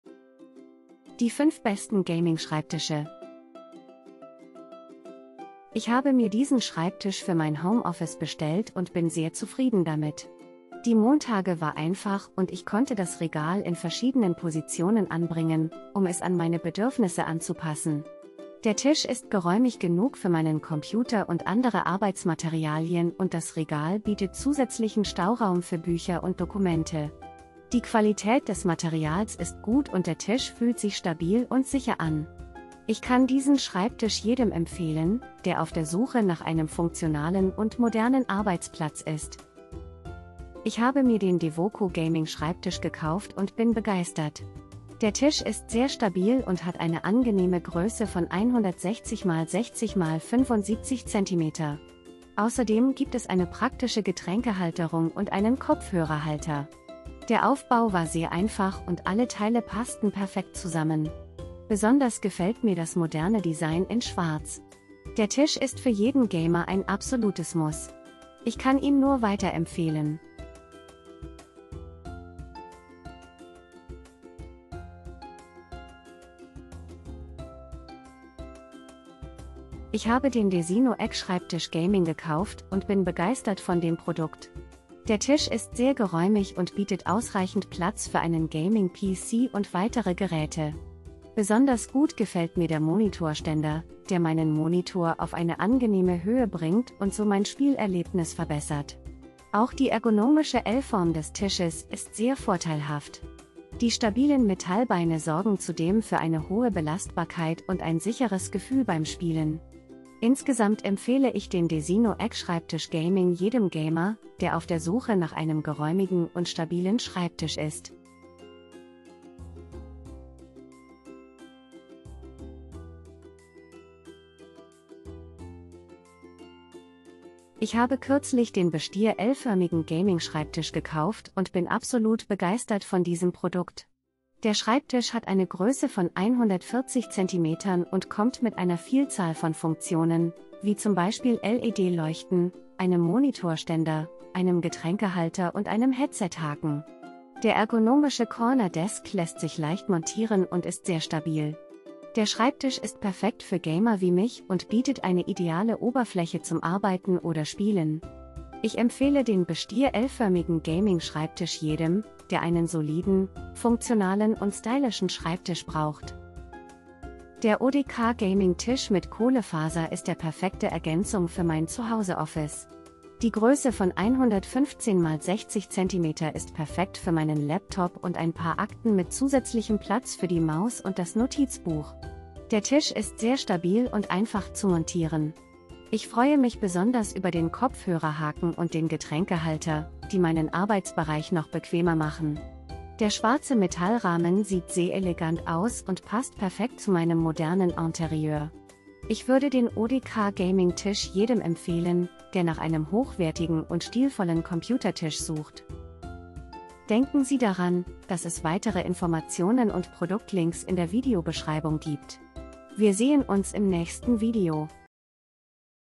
【GAMING DESK】Die 5 besten Gaming-Schreibtische
🙂 Alle Produkte

✋  Hier sind die 5 besten gaming-schreibtische auf dem Markt.

🔽🔽🔽 KAUFEN-LINKS GAMING DESK 🔽🔽🔽

1) HOMIDEC Schreibtisch, Computertisch mit Bücherregal, Studiencomputer-Laptoptisch mit 4 Tier DIY Lagerregalen Schreiben Tisch für Home Office Schlafzimmer 160x60x110cm

2) Devoko Gaiming Tisch 160 cm Gaming Schreibtisch Gamer Computertisch Ergonomischer PC Schreibtisch mit Getränkehalter und Kopfhörerhalter T-förmiger (Schwarz, 160 x 60 x 75 cm)

3) DESINO Eckschreibtisch Gaming 150 x 120 cm, Ergonomic Gamer Schreibtisch l Form, Groß Pc Ecktisch Computertisch Mit Monitorständer, Stabil Metall-Beine Eck Tisch, Schwarz

4) Bestier L förmigen Gaming Schreibtisch Eckschreibtisch mit LED Leuchten Monitorständer 140CM Gaming Computer Eckschreibtisch mit Getränkehalter Headset Haken Ergonomischer Corner Desk

5) ODK Gaming Tisch mit Kohlefaser 115 x 60cm, Computertisch mit Kopfhörerhaken und Getränkehalter, ergonomischer PC Gaming Tisch, Stabiler Metallrahmen, schwarz

* Zeitindikatoren (Timestamps) *

00:00 - Intro
00:04 - 1) HOMIDEC Schreibtisch, Computertisch mit Bücherregal,...
00:39 - 2) Devoko Gaiming Tisch 160 cm Gaming Schreibtisch Gamer...
01:18 - 3) DESINO Eckschreibtisch Gaming 150 x 120 cm, Ergonomic...
02:06 - 4) Bestier L förmigen Gaming Schreibtisch Eckschreibtisch mit...
02:46 - 5) ODK Gaming Tisch mit Kohlefaser 115 x 60cm,...
03:25 - Outro